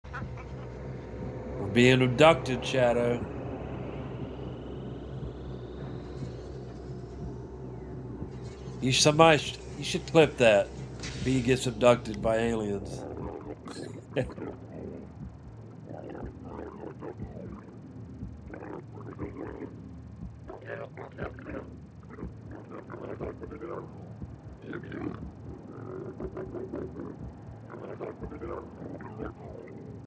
Alien Abduction on Twitch
B gets abducted by Aliens